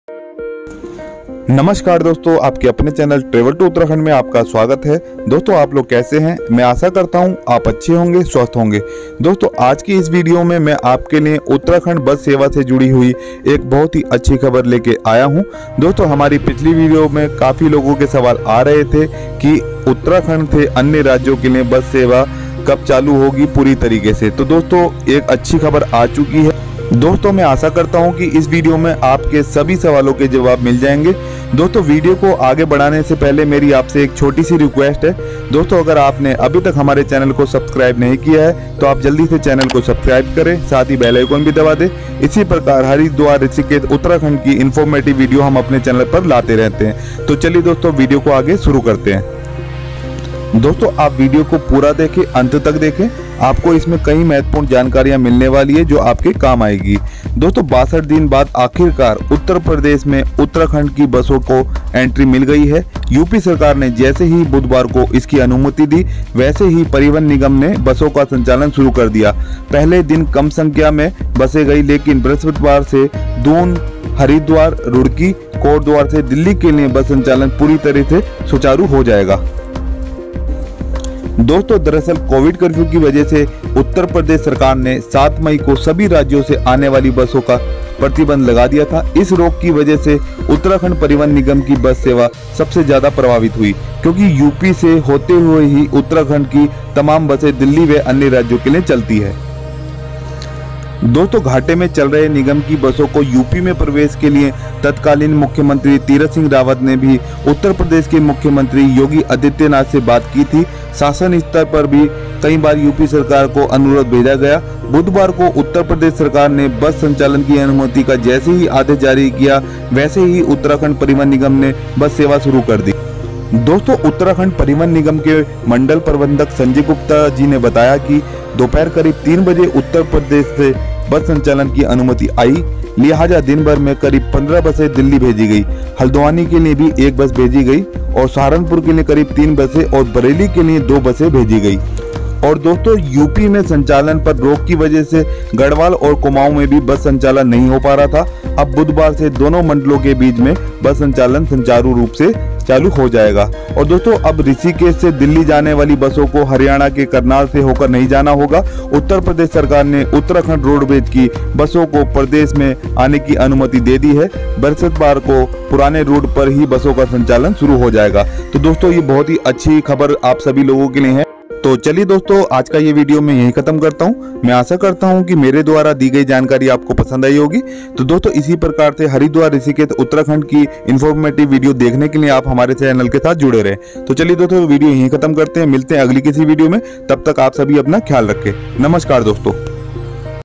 उत्तराखंड से सभी राज्यों की बस सेवा शुरू हुई। क्या अब आप उत्तराखंड से उत्तर प्रदेश जा पाएंगे
Hello friends

                I am Amit Chandra walcome to my YouTube channel travel to uttarakhand...

Friends is video mein Ham aapko Bata rahe hain Uttrakhand se sabhi rajyon Ke liye bus service shuru ho chuki hai video kafi achcha banaa hai video ko aap pura dekhen aur channel ko subscribe jarur Karen
                                          Travel To Uttarakhand